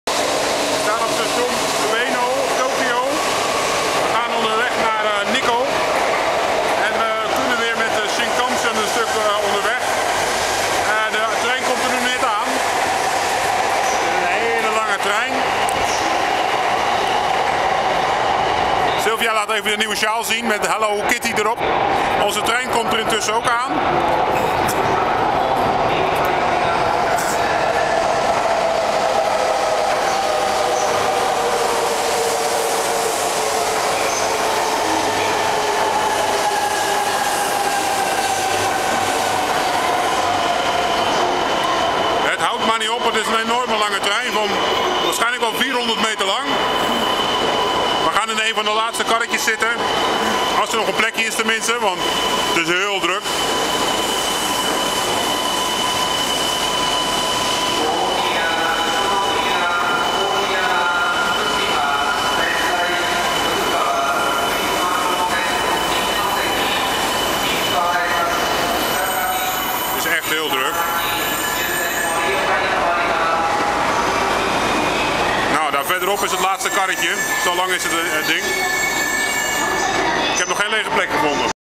400 meter trein stopt voor onze neus
We laten zien hoe lang de treinen in Japan zijn op het station van Ueno. Syl showt terloops nog even haat Hello Kitty sjaal